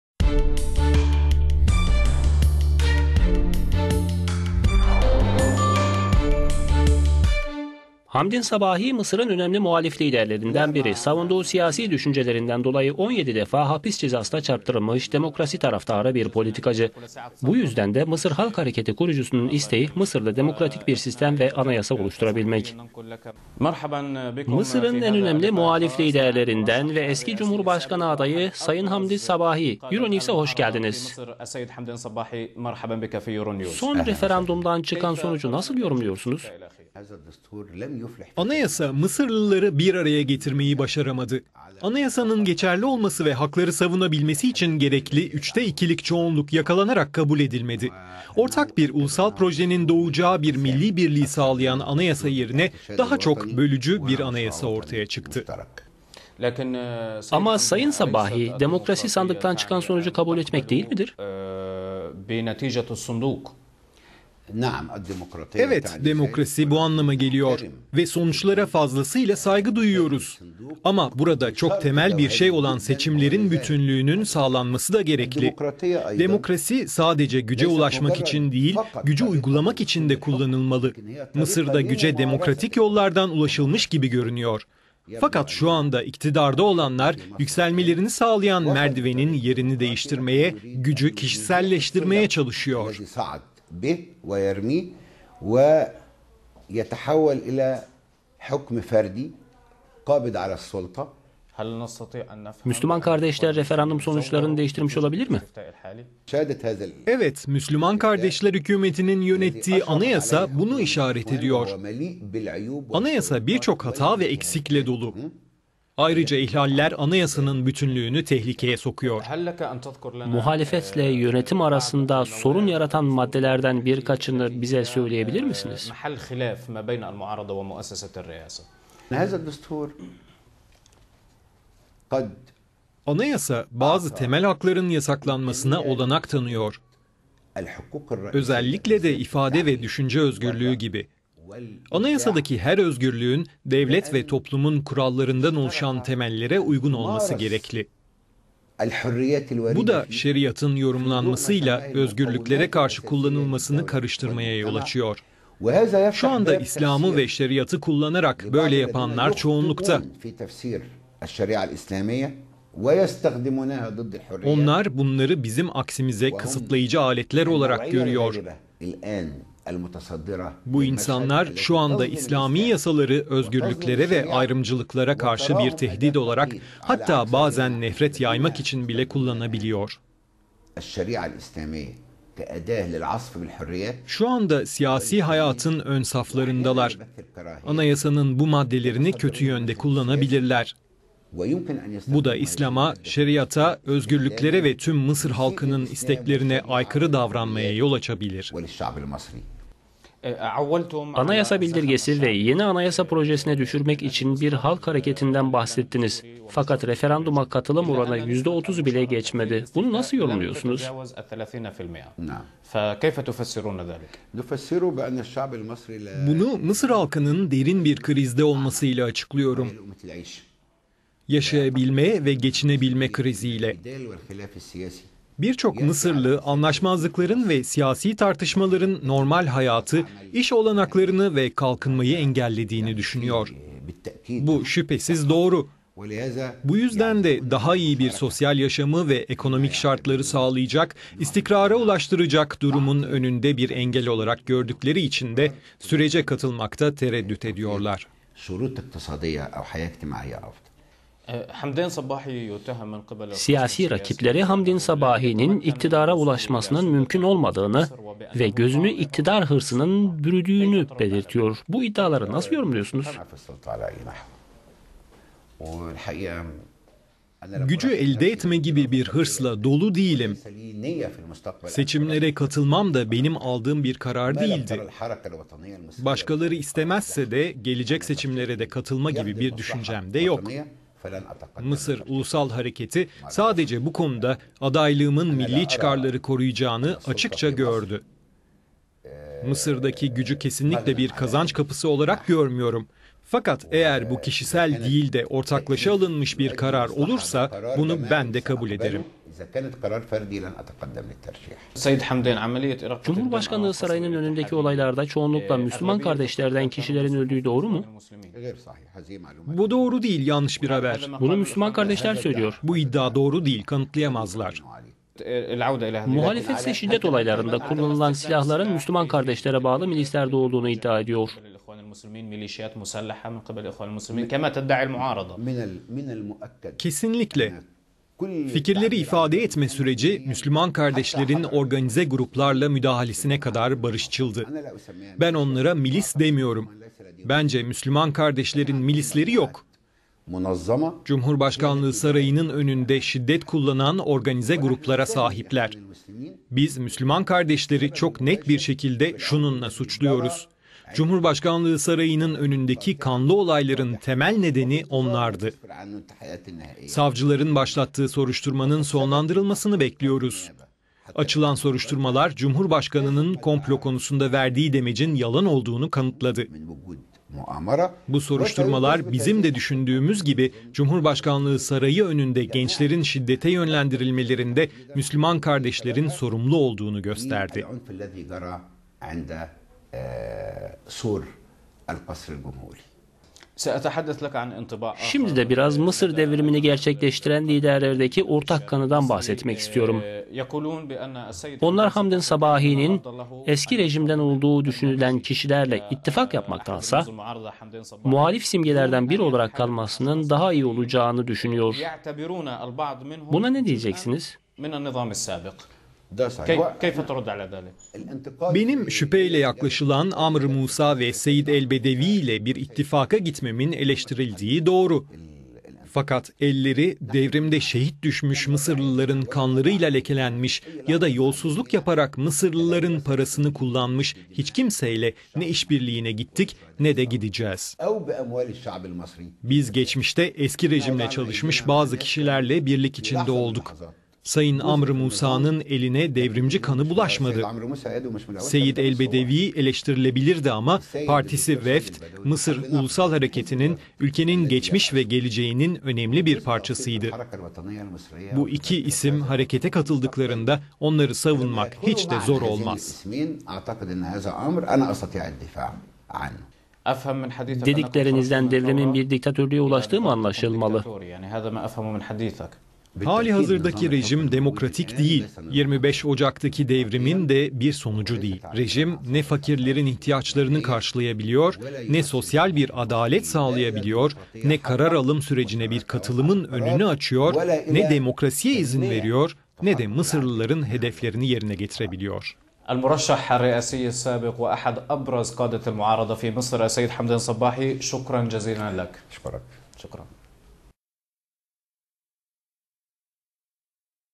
euronews interview - Mısır: "İktidar, elindeki gücü kişiselleştirmeye çalışıyor"
Hamdin Sabahi Mısır'ın önemli muhalif liderlerinden biri. Demokrasi taraftarı bir politikacı savunduğu siyasi düşüncelerinden dolayı 17 kere hapis cezasına çarptırılmış. Bu yüzden de Mısır Halk Hareketi Kurucusu'nun isteği ülkede demokratik bir sistem ve anayasa oluşturabilmek.

euronews:
"Mısır'ın en önemli muhalif liderlerinden ve eski cumhurbaşkanı adayı sayın Hamdin Sabahi euronews'a hoş geldiniz. Son referandumdan çıkan sonucu nasıl yorumluyorsunuz?"

Hamdin Sabahi:
"Anayasa Mısırlıları bir araya getirmeyi başaramadı. Anayasanın geçerli olması ve hakları savunabilmesi için gerekli üçte ikilik çoğunluk yakalanarak kabul edilmedi. Ortak bir ulusal projenin doğacağı bir milli birliği sağlayan anayasa yerine daha çok bölücü bir anayasa ortaya çıktı."

euronews:
"Ama sayın Hamdin, demokrasi sandıktan çıkan sonucu kabul etmek değil midir?"

Hamdin Sabahi:
"Evet demokrasi bu anlama geliyor. Ve sonuçlara fazlasıyla saygı duyuyoruz. Ama burada çok temel bir şey olan seçimlerin bütünlüğünün sağlanması da gerekli. Demokrasi sadece güce ulaşmak için değil gücü uygulamak için de kullanılmalı. Mısır'da güce demokratik yollardan ulaşılmış gibi görünüyor. Fakat şu anda iktidarda olanlar yükselmelerini sağlayan merdivenin yerini değiştirmeye, gücü kişiselleştirmeye çalışıyor."

euronews:
"Müslüman Kardeşler'in referandum sonuçları üzerinde değişiklikler yaptığını söyleyebilir miyiz?"

Hamdin Sabahi:
"Evet, Müslüman Kardeşler hükümetinin yönettiği anayasa bunu işaret ediyor. Anayasa birçok hata ve eksikle dolu. Ayrıca ihlaller anayasanın bütünlüğünü tehlikeye sokuyor."

euronews:
"Muhalefetle yönetim arasında sorun yaratan maddelerden birkaçını bize söyleyebilir misiniz?"

Hamdin Sabahi:
"Anayasa bazı temel hakların yasaklanmasına olanak tanıyor. Özellikle de ifade ve düşünce özgürlüğü gibi. Anayasaya'daki her özgürlüğün, devlet ve toplumun kurallarından oluşan temellere uygun olması gerekiyor. Bu da Şeriat'ın yorumlanması ile özgürlüklere karşı kullanılmasını karıştırmaya neden oluyor. Şu anda İslam'ı ve Şeriat'ı kullanarak böyle yapanlar çoğunlukta. Onlar bunları bizim aksimize kısıtlayıcı aletler olarak görüyor. Bu insanlar şu anda İslami yasaları özgürlüklere ve ayrımcılıklara karşı bir tehdit olarak, hatta bazen nefret yaymak için bile kullanıyor. Şu anda siyasi hayatın ön saflarındalar. Anayasanın bu maddelerini kötü yönde kullanabilirler. Bu da İslam'a, Şeriat'a, özgürlüklere ve tüm Mısır halkının isteklerine aykırı davranmaya yol açabilir."

euronews:
"Anayasa bildirgesi ve yeni anayasa projesini düşürmek için bir halk hareketinden bahsettiniz. Fakat referanduma katılım oranı yüzde otuzu bile geçmedi. Bunu nasıl yorumluyorsunuz?"

Hamdin Sabahi:
"Bunu Mısır halkının derin bir krizde olması ile açıklıyorum. Yaşayabilme ve geçinebilme krizi ile. Birçok Mısırlı anlaşmazlıkların ve siyasi tartışmaların normal hayatı, iş olanaklarını ve kalkınmayı engellediğini düşünüyor. Bu şüphesiz doğru. Bu yüzden de daha iyi bir sosyal yaşamı ve ekonomik şartları sağlayacak, istikrara ulaştıracak durumun önünde bir engel olarak gördükleri için de sürece katılmakta tereddüt ediyorlar.

euronews
"Siyasi rakipleri Hamdin Sabahi'nin iktidara ulaşmasının mümkün olmadığını ve gözünü iktidar hırsının bürüdüğünü belirtiyor. Bu iddiaları nasıl yorumluyorsunuz?"

Hamdin Sabahi:
"Gücü elde etme gibi bir hırsla dolu değilim. Seçimlere katılmam da benim aldığım bir karar değildi. Başkaları istemezse de gelecek seçimlere de katılma gibi bir düşüncem de yok. Mısır Ulusal Hareketi, sadece bu konuda, adaylığımın milli çıkarları koruyacağını açıkça gördü. Mısır'daki gücü kesinlikle bir kazanç kapısı olarak görmüyorum. Fakat eğer bu kişisel değil de ortaklaşa alınmış bir karar olursa, bunu ben de kabul ederim."

euronews:
"Cumhurbaşkanlığı Sarayı'nın önündeki olaylarda çoğunlukla Müslüman Kardeşler'den kişilerin öldüğü doğru mu?"

Hamdin Sabahi:
"Bu doğru değil, yanlış bir haber."

euronews
"Müslüman Kardeşler bunu söylüyor."

Hamdin Sabahi:
"Bu iddia doğru değil, kanıtlayamazlar."